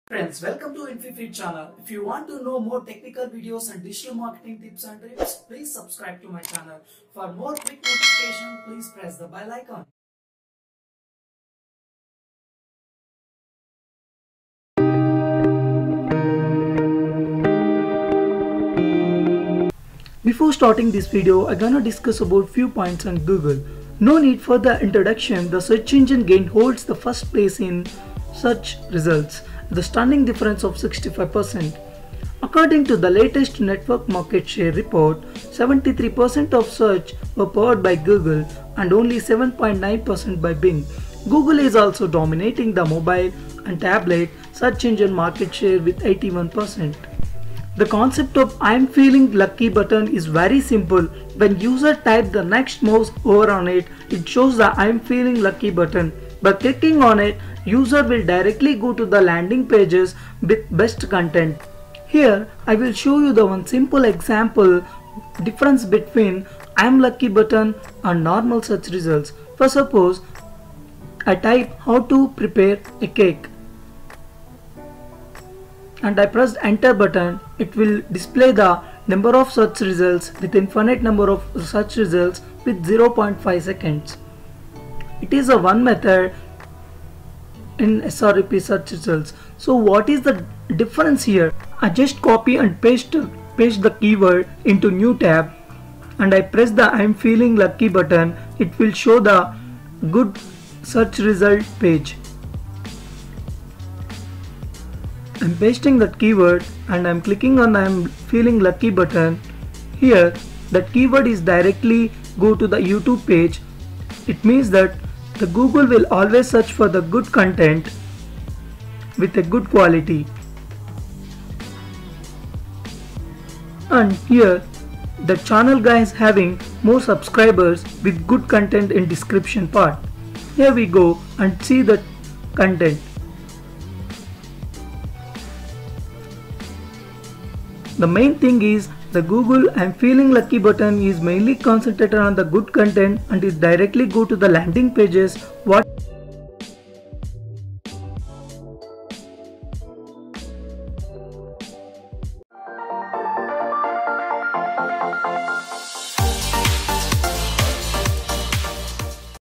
Google : i 'm Feeling Lucky
The Main concept of "I'm Feeling Lucky" button is very simple When the user types the text and mouse over on it, the user will be landed directly on the first search result.

Google Secerts :

1.No need for further introductions.
The search engine giant holds the first place in search with a stunning difference of 65%.

2. According to the latest net market share, report73% of searches were powered by Google and only 7.91% by Bing.

3. Google is also dominating the mobile/tablet search engine market share with 81%!